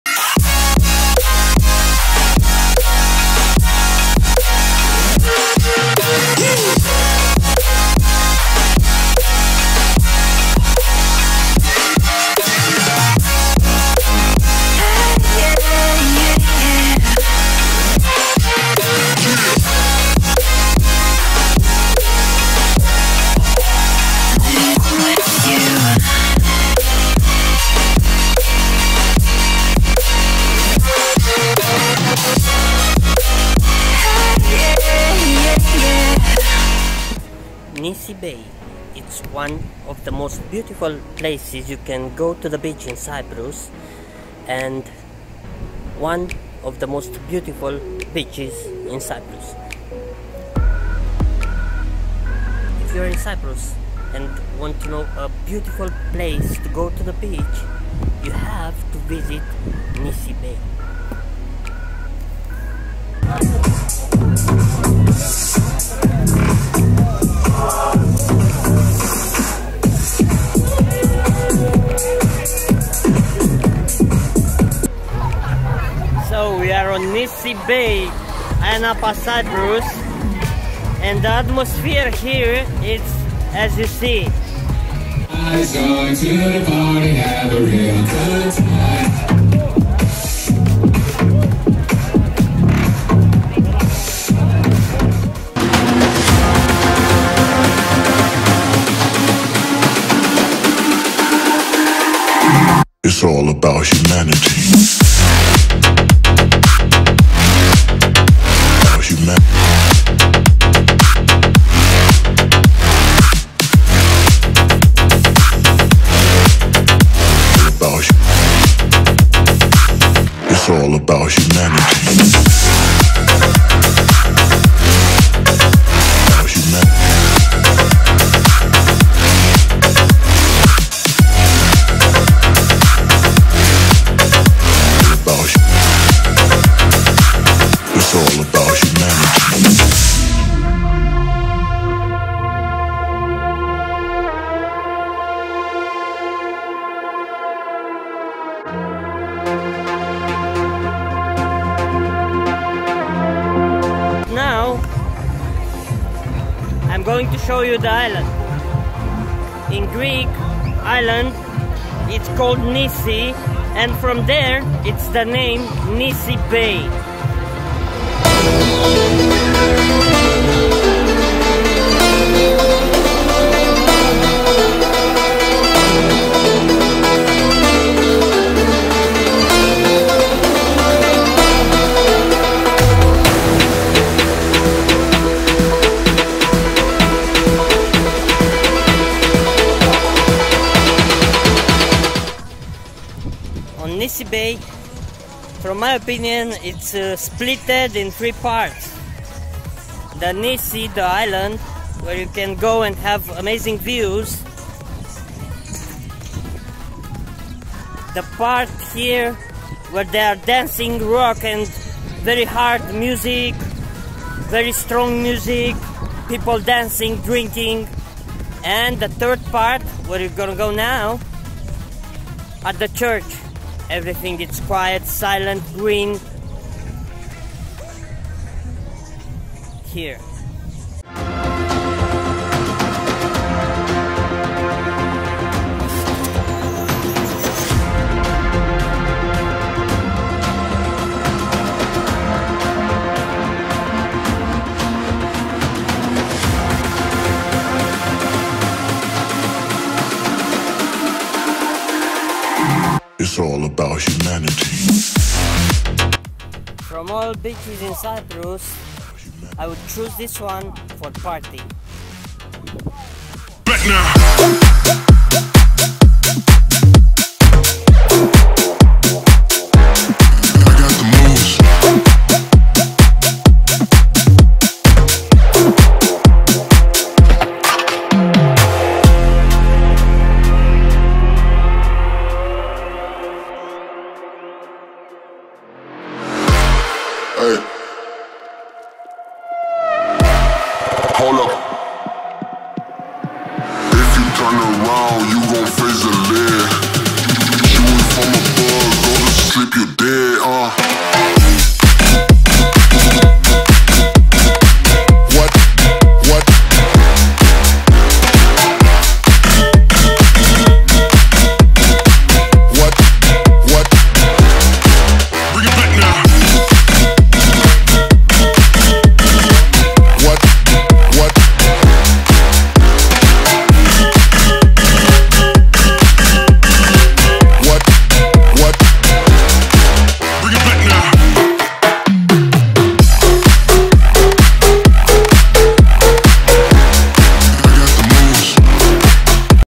Nissi Bay Cyprus Ayia Napa
Ayia Napa Nissi Strand, Ayia Napa Nissi Beach is one of the most popular beaches in Cyprus and you can visit it if you are in Ayia Napa.
Nissi Beach it is famous for the beach club and the parties that are happening there.